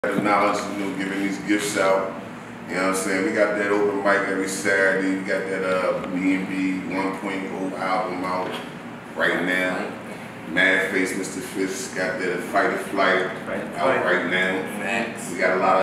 BNB Ent inc Dropping Knowledge about Artists
BNB ent Inc Came by and dropped so many pointers about the Battle Rap scene , djs , the ethics of running a business , and having partnerships in the community.